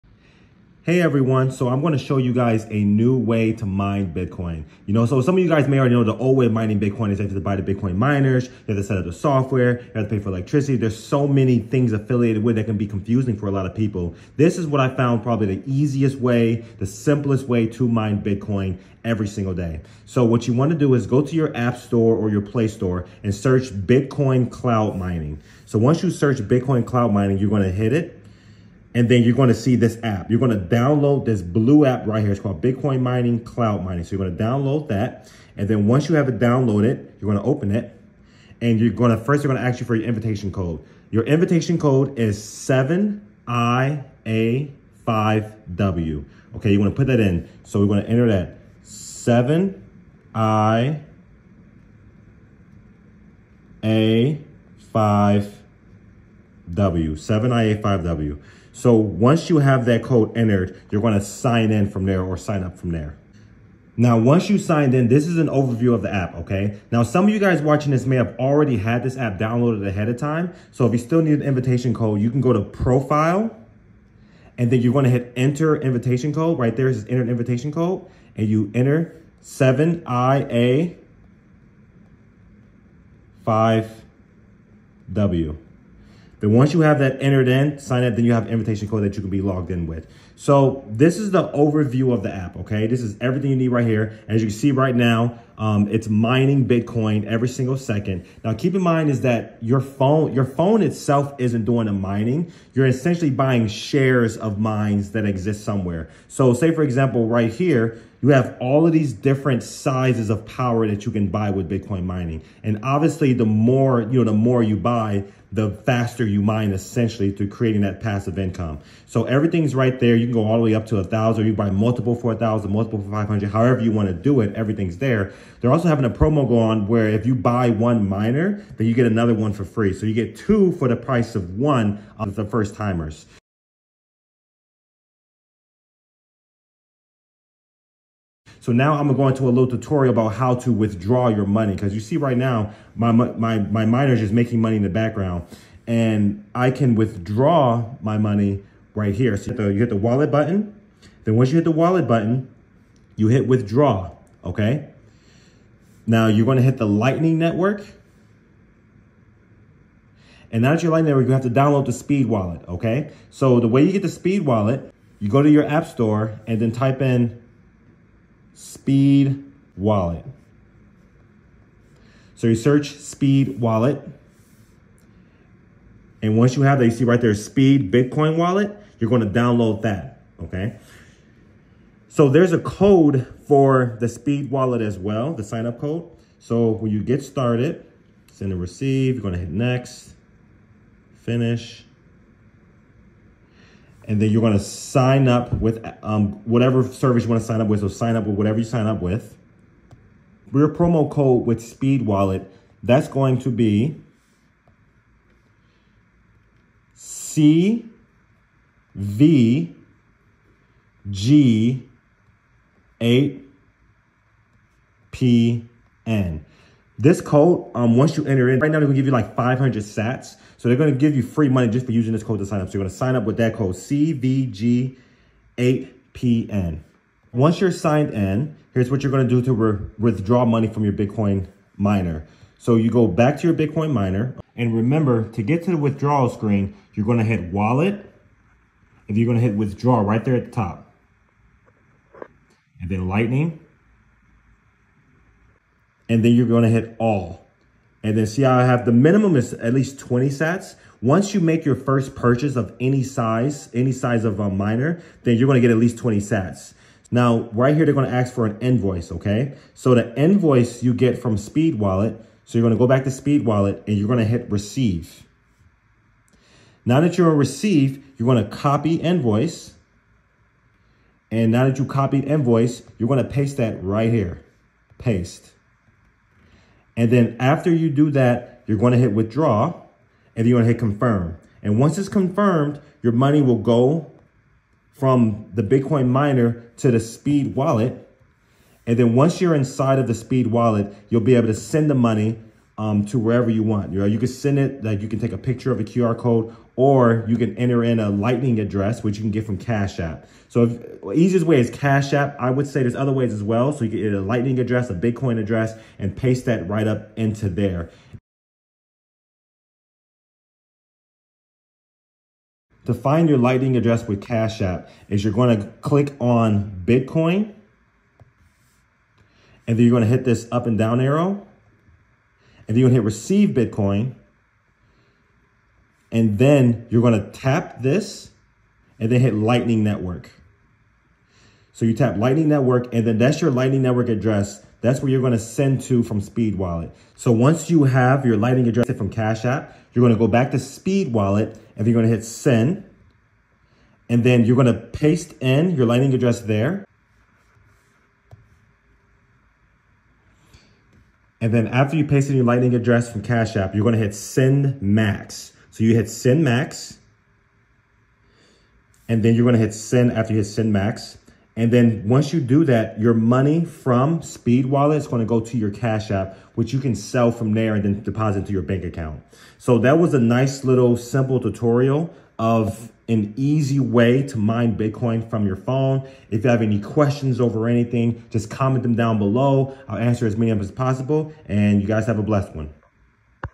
A New Way To Mine Bitcoin: Bitcoin Cloud Mining App Tutorial + Withdrawal via Speed Bitcoin Wallet
Bitcoin Cloud Mining App Invitation Code 👉🏿 7iA5W

Download Bitcoin Cloud Mining App

“Click Allow Paste when signing up for Speed Bitcoin Wallet”

Unlock the secret to earning passive income with The Bitcoin Mining • Cloud Mining App! In this video, I’ll teach you how to mine Bitcoin without the need for expensive mining hardware. Using the Bitcoin Cloud Mining App, you can start mining Bitcoin with just your phone – and the best part? The app is completely FREE to download!

I’ll also guide you step-by-step on how to withdraw your earnings through the Speed Bitcoin Wallet and send your funds directly to your Cash App for easy access to your money.

💡 What You’ll Learn in This Video:

1️⃣ How to set up and start mining with Bitcoin Cloud Mining App.

2️⃣ How to withdraw your newly mined Bitcoin into Speed Bitcoin Wallet.

👉 Ready to start your journey toward financial freedom? Watch now and start earning Bitcoin today!